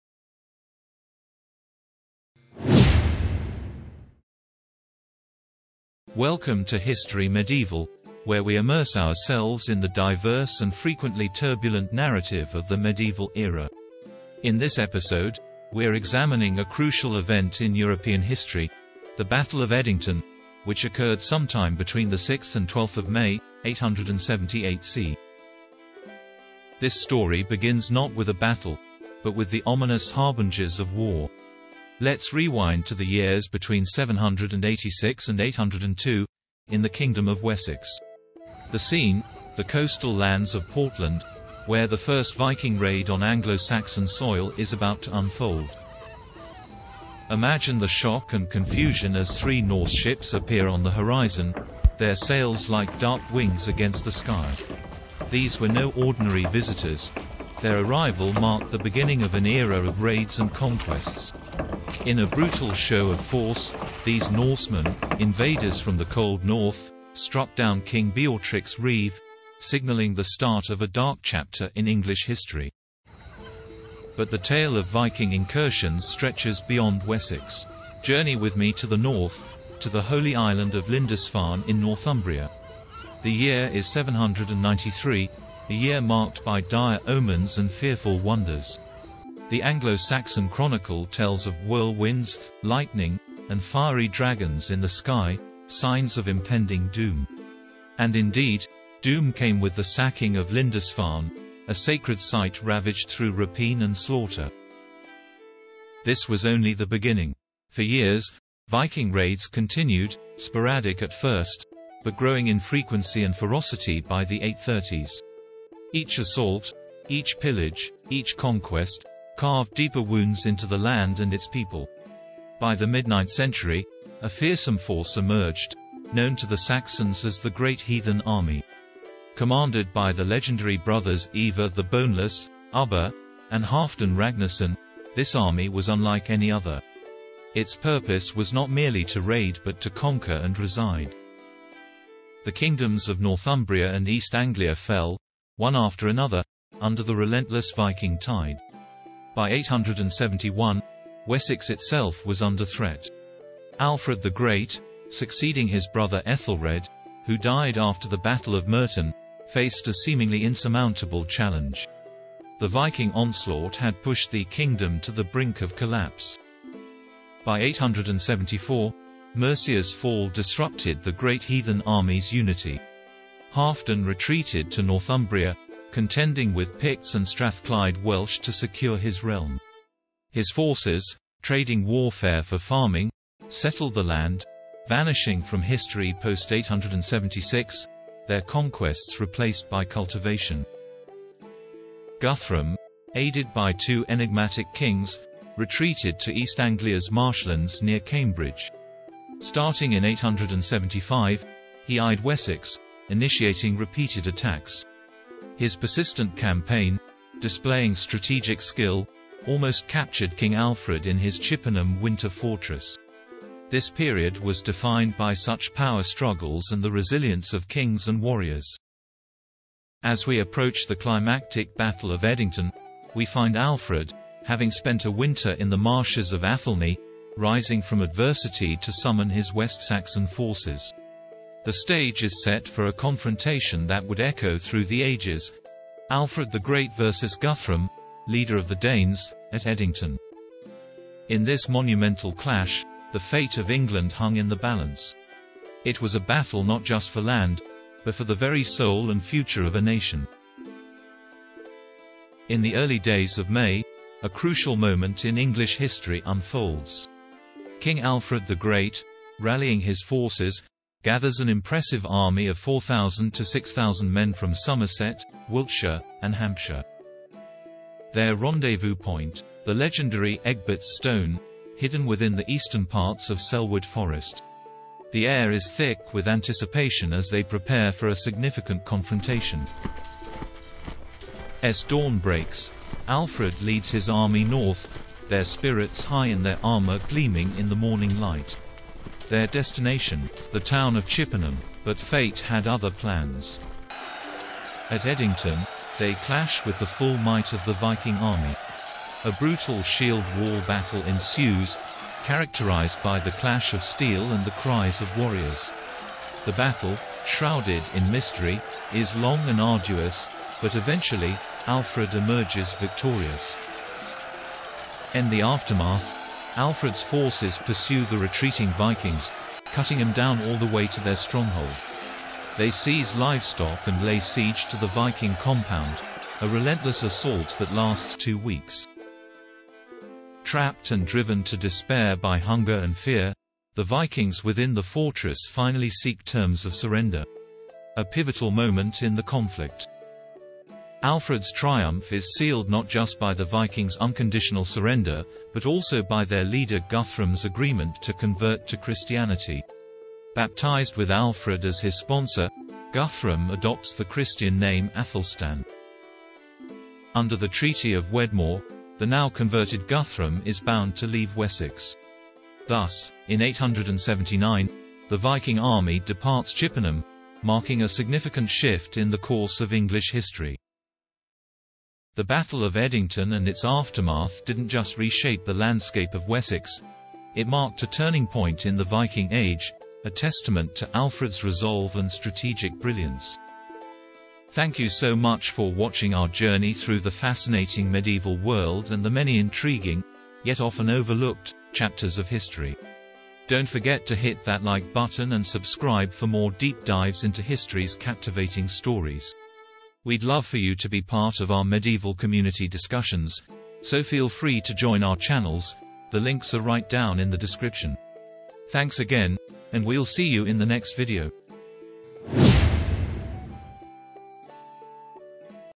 The Battle of Edington, 878 AD
In this captivating episode, we delve into the pivotal Battle of Edington, a clash in 878 AD that not only shaped the course of English history but also marked the beginning of the end of Viking dominance in England. Here, the West Saxon king Alfred the Great, emerging from his strategic retreat to the Somerset Levels, orchestrated a decisive victory against the formidable Viking leader Guthrum. This triumph not only secured Alfred's legacy but also led to the Treaty of Wedmore, which paved the way for the eventual unification of England. Join us as we explore the strategies, the heroics, and the profound consequences of this legendary battle.